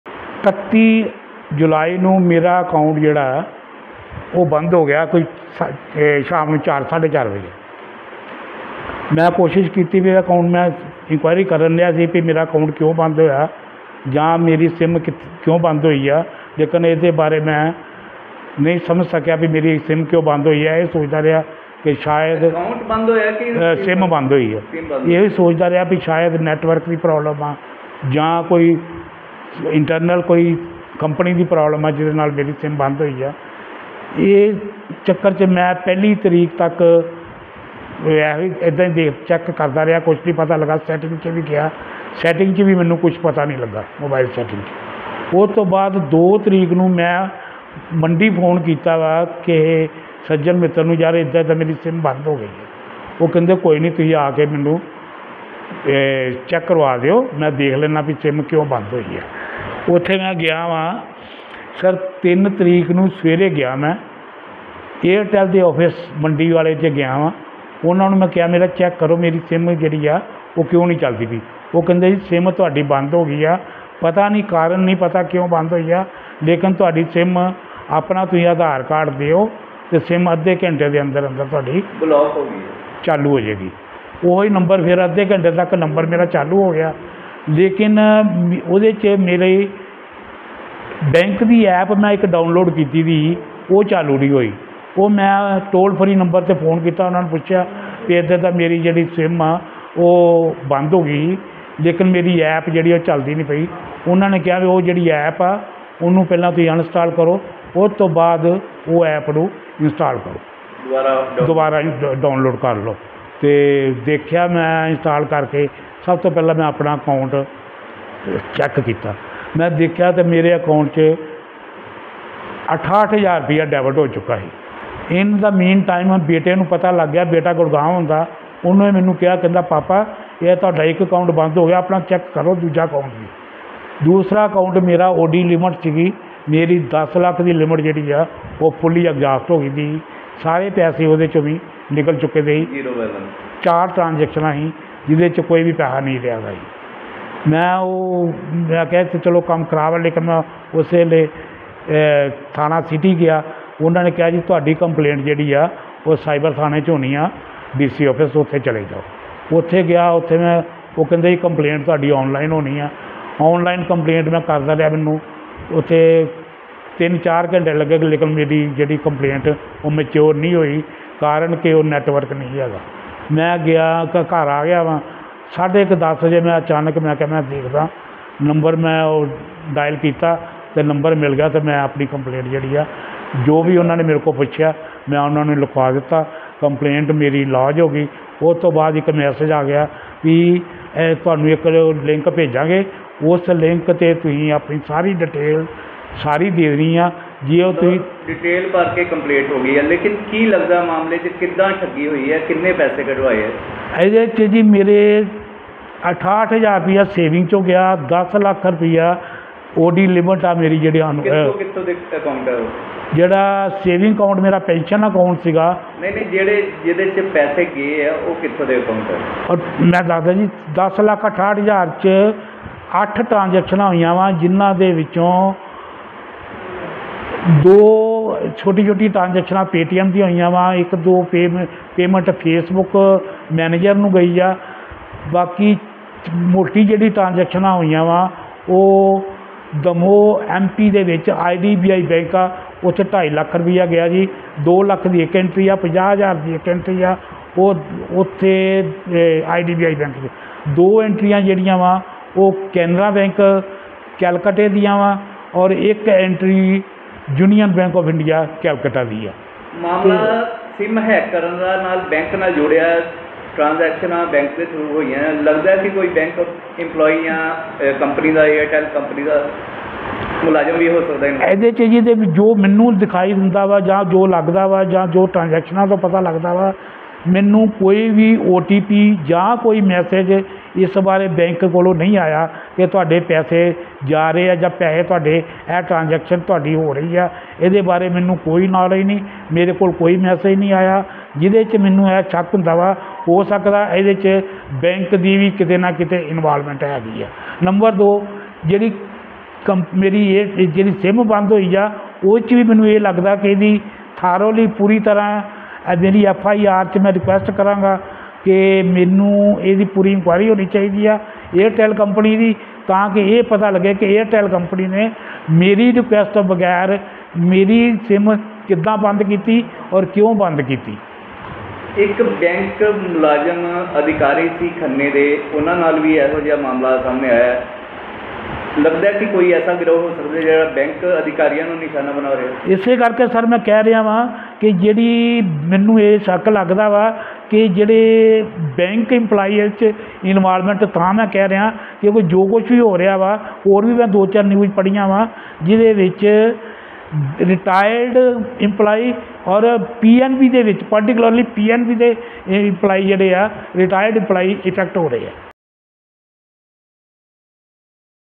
Loot In Gurdaspur | ਬੈਂਕ 'ਚ ਕੰਮ ਕਰ ਚੁੱਕੇ ਮੁਲਾਜ਼ਮ ਨਾਲ ਹੋਈ ਮਾੜੀ, ਖਾਤੇ 'ਚੋਂ ਉੱਡਾਏ 11 ਲੱਖ
ਗੁਰਦਾਸਪੁਰ ਦੀ ਇੰਪਰੂਵਮੈਂਟ ਟਰੱਸਟ ਕਲੋਨੀ ਸਕੀਮ ਨੰਬਰ ਪੰਜ ਵਿੱਚ ਰਹਿਣ ਵਾਲੇ ਪੰਜਾਬ ਨੈਸ਼ਨਲ ਬੈਂਕ ਦੇ ਸੇਵਾਮੁਕਤ ਸੀਨੀਅਰ ਸਿਟੀਜ਼ਨ ਹੀਰਾ ਲਾਲ ਅਗਰਵਾਲ ਨੇ ਦੱਸਿਆ ਕਿ 31 ਜੁਲਾਈ ਨੂੰ ਸ਼ਾਮ 4.40 ਵਜੇ ਉਨ੍ਹਾਂ ਨੂੰ ਕਿਸੇ ਅਣਪਛਾਤੇ ਮੋਬਾਈਲ ਨੰਬਰ ਤੋਂ ਕ੍ਰੈਡਿਟ ਕਾਰਡ ਰੱਦ ਕਰਨ ਦਾ ਵਟਸਐਪ ਸੁਨੇਹਾ ਮਿਲਿਆ।